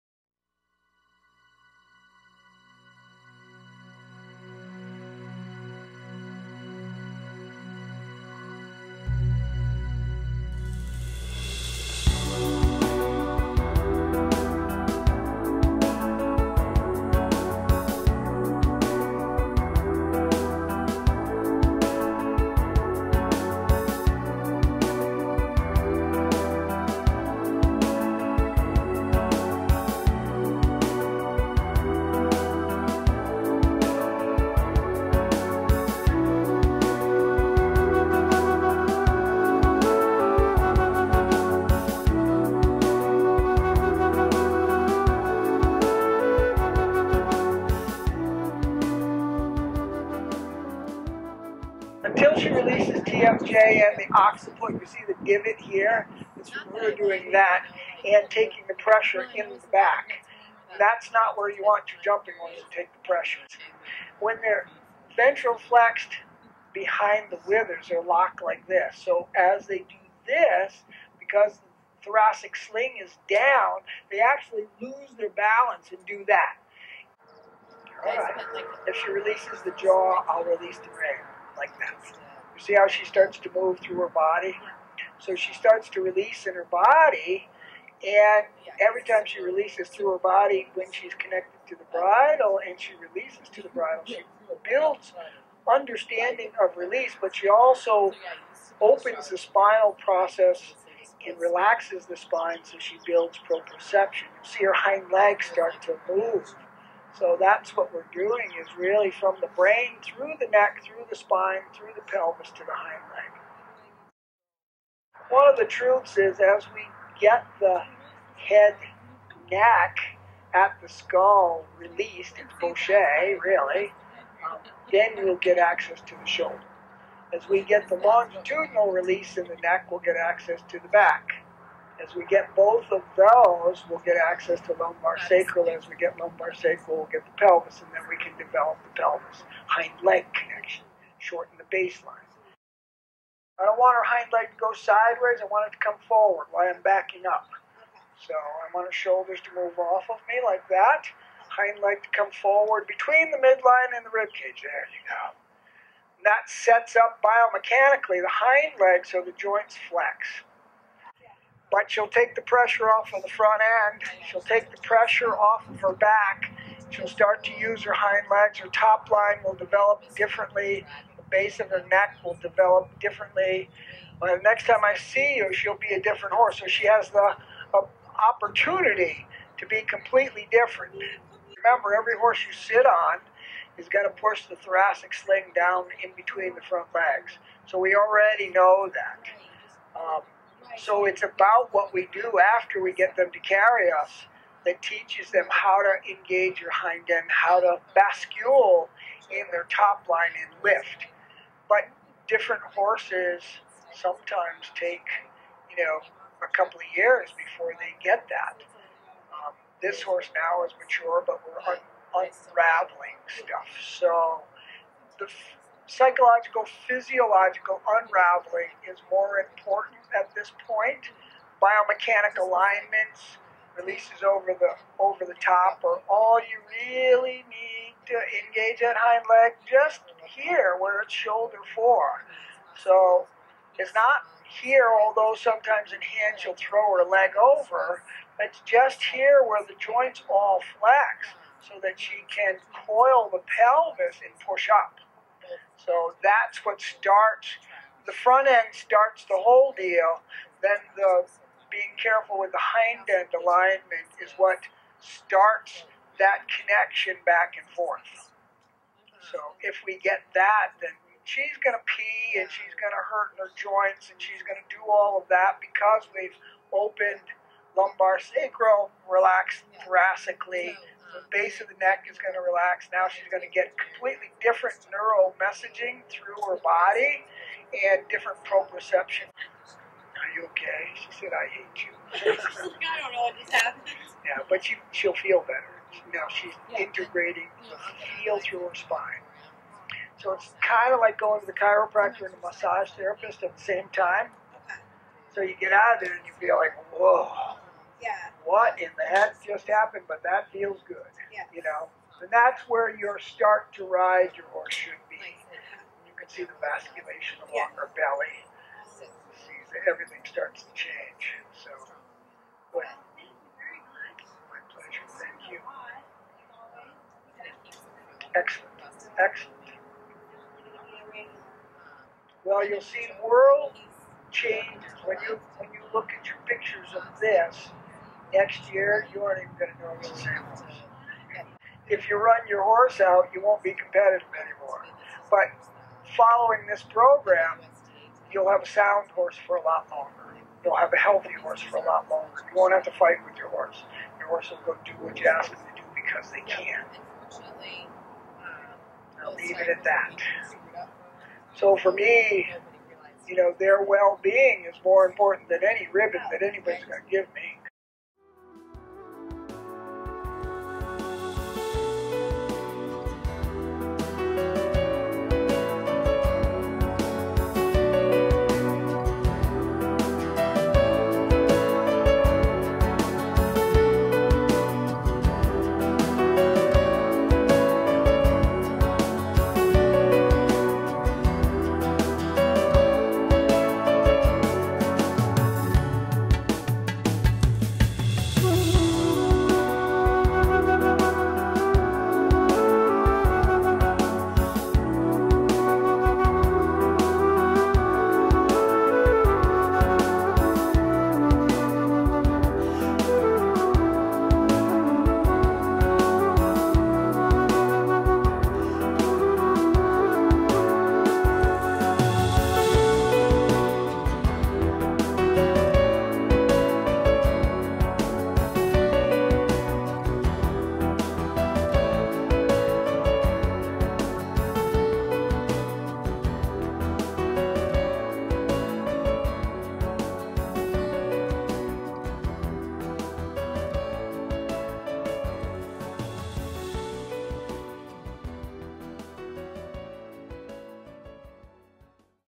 Mark Russell: a little video memory of Mark working with a blocked and braced jumper.
Moments from a Mark Russell clinic in 2016. Mark works with a competitive jumper who had trouble with bracing and stiffness. This is a brief moment in time with Mark seven years after his loss. This is not a complete lesson and is only intended to provide a memory of him for those who loved him. For more detailed instructions visit naturaldressage.com or Mark Russell Natural Dressage on Facebook.